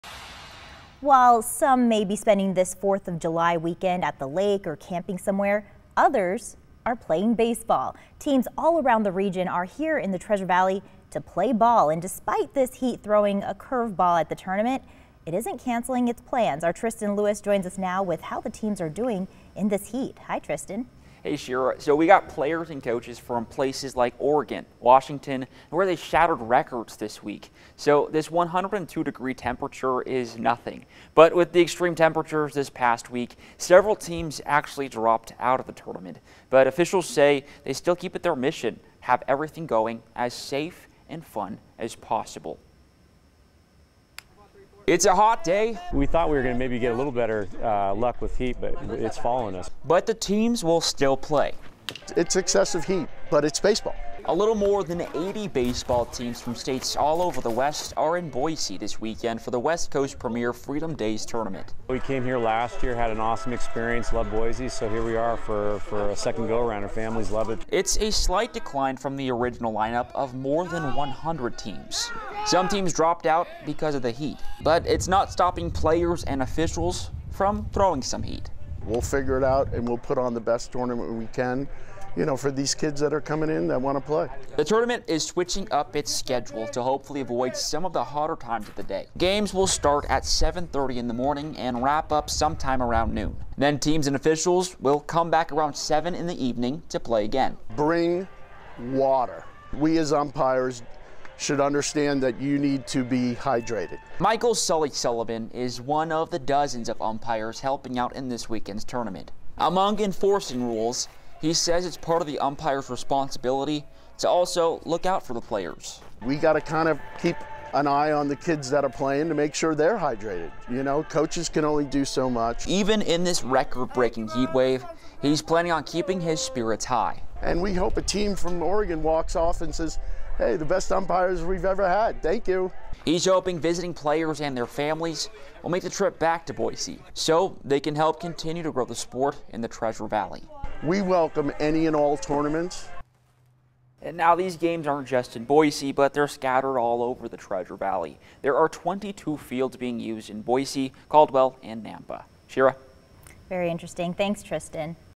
Play ball: West Coast baseball tournament continues in Treasure Valley despite heat
The West Coast Premier Freedom Days tournament brought teams from Oregon, Washington, eastern Idaho, Nevada and more to the area.